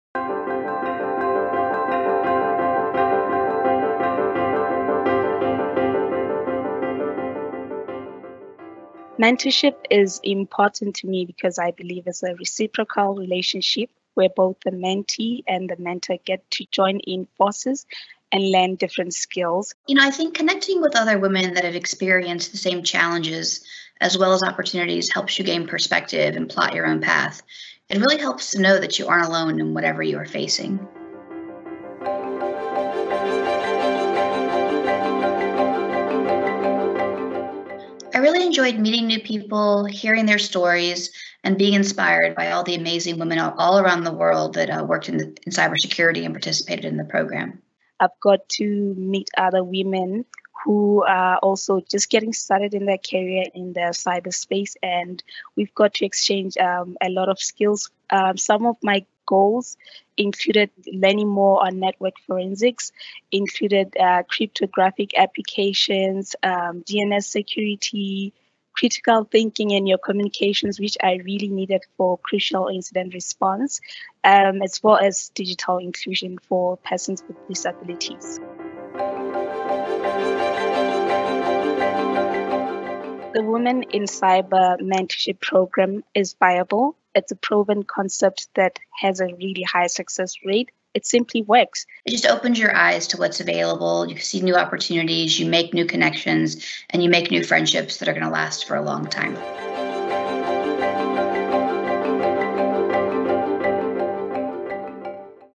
ITU-D Women in Cyber Mentorship Programme: Testimonials from Fiona Alexander and Angela Matlapeng
Hear from a mentor and mentee of the 2021 edition of ITU’s Women in Cyber Mentorship Programme, on the importance of mentorship, their experience in the programme, and how it has impacted their lives.
(deadline: 31 March 2022)